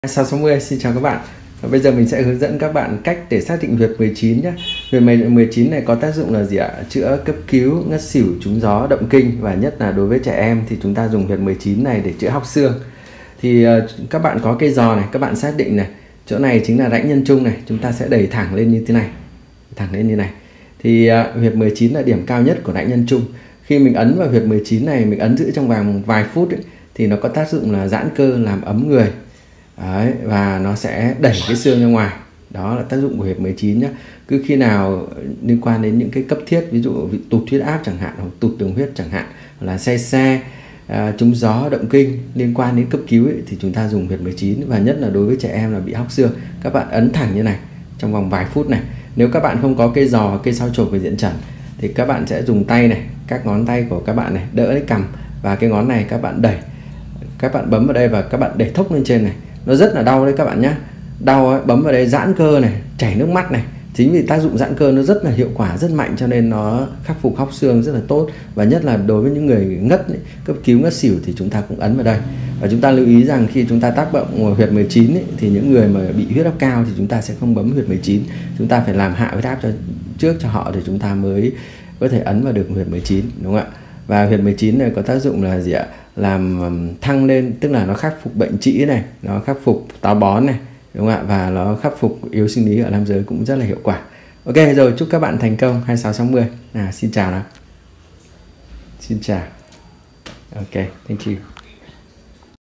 Cách xác định huyệt 19 chữa hóc xương
Hóc xương. !
Chẳng may khi bản thân mình hoặc con nhỏ bọ hóc xương cá hoặc dị vật. Bạn hãy nhanh tay ấn giữ huyệt 19 của Diện Chẩn Bùi Quốc Châu trong vài phút, sẽ có kết quả tức thì.

Cách xác định huyệt 19: cực kỳ đơn giản, nó nằm ở điểm cao nhất của rãnh nhân trung. Nếu có cây dò bạn đẩy từ phía dưới của rãnh nhân trung chạm vào vách ngăn lỗ mũi. Nếu không có cây dò thì bạn dùng ngón tay cái bấm vào điểm này, các ngón còn lại đỡ cằm.

Huyệt 19 còn có tác dụng cấp cứu ngất xỉu, khai thông bế tắc, chữa nấc, cắt cơn đau nhanh (đặc biệt là đau bao tử), động kinh, bệnh tim,...

Bị trĩ lòi rom, táo bón ấn huyệt này. đi trơn tru luôn.

Nếu ông nào yếu sinh lý, gió thổi mãi mà cờ không phất được thì cũng ấn vào đây vài phút nhé. Cẩn thận. phất mạnh quá không chừng! ^^

Khi tụt huyết áp, tụt đường huyết cũng đừng quên huyệt này vì nó làm ấm người & tăng huyết áp cũng rất là nhanh. Chính vì thế huyệt này không dùng cho người huyết áp cao. Nếu muốn dùng phải hạ áp trước. Huyệt này cũng không dùng cho phụ nữ đang mang thai, nhưng nếu đến ngày sinh nở ấn vào đây lại giúp bà mẹ sinh con dễ dàng và nhanh hơn.

Đây đơn giản như là một mẹo sinh tồn, một kỹ năng sống mà ai cũng cần.

Chúc bạn thành công nhé!
26-60!
Vũ Văn Hội, HN-18/08/20